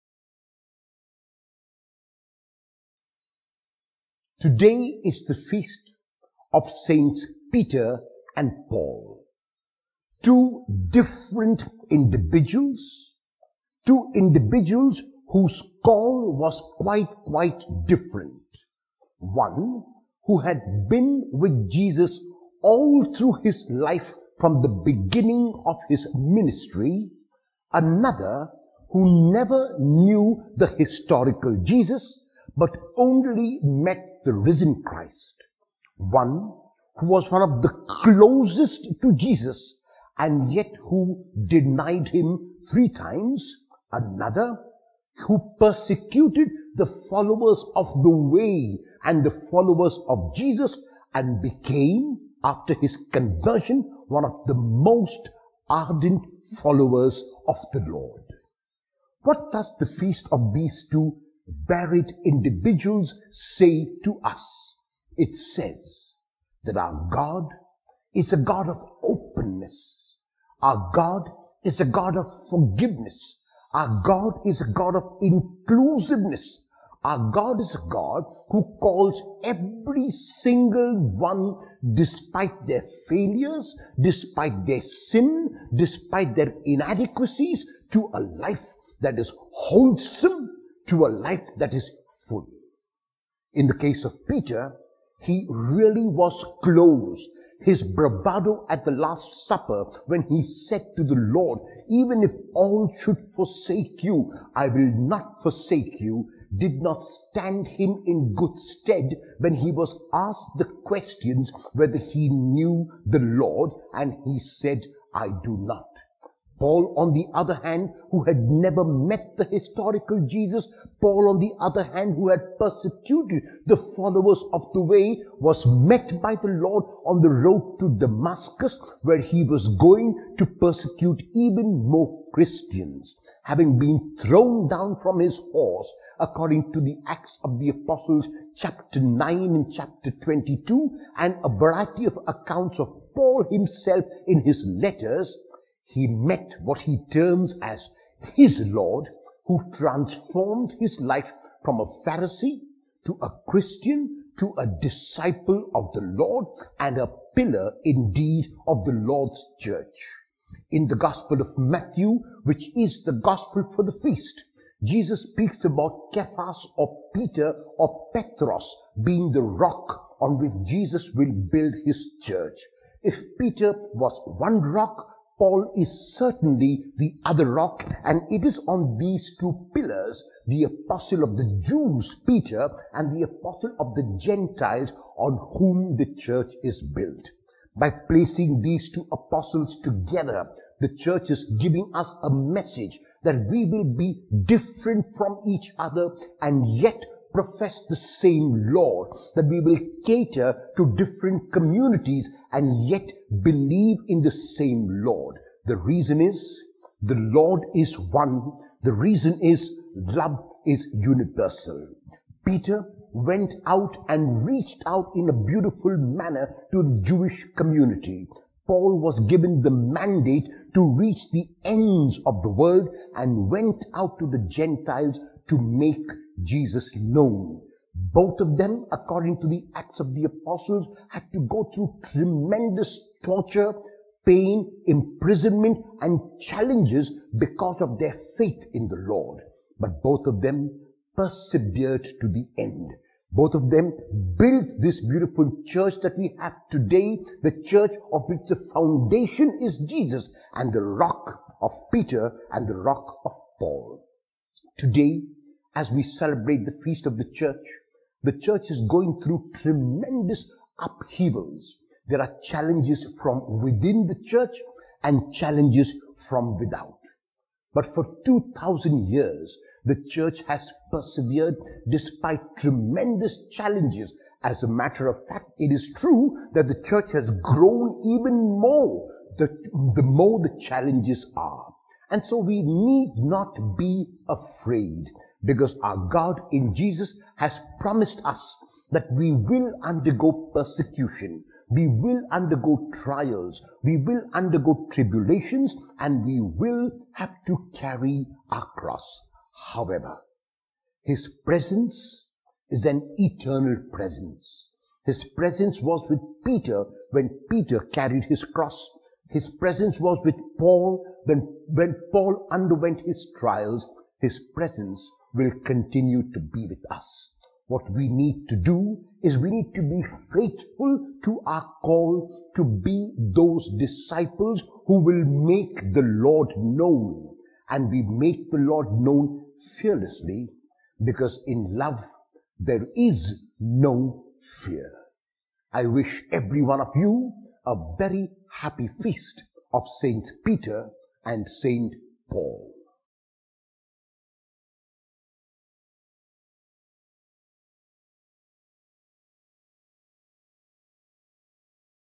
Tuesday, June 29, 2021 Homily
Today we celebrate the fidelity of Peter and Paul, sinners like us all. Initially, they were both found wanting. When they eventually repented, they were forgiven by God in Christ. Though they faced persecution, their commitment to Christ gave them the courage they needed. Their victory is evidence that the truth will overcome untruth, light will overcome darkness and life will overcome death. Their victory is evidence that we shall indeed overcome.